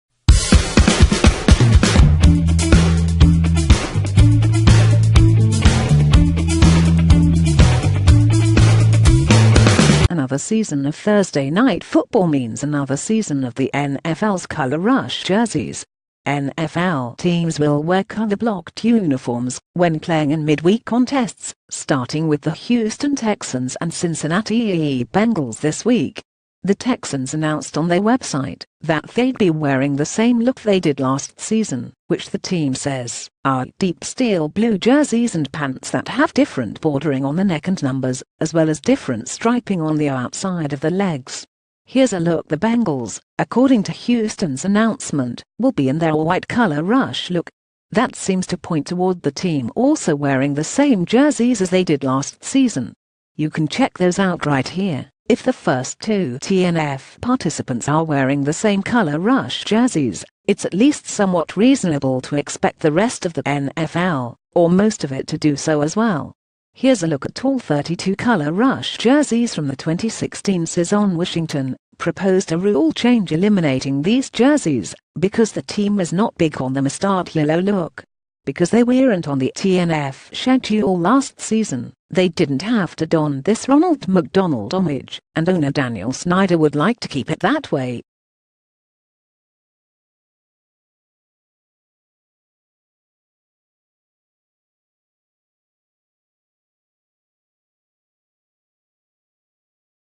[Daily News] Look: texans, bengals will wear color rush jerseys for 'thursday night football'
Look: texans, bengals will wear color rush jerseys for 'thursday night football'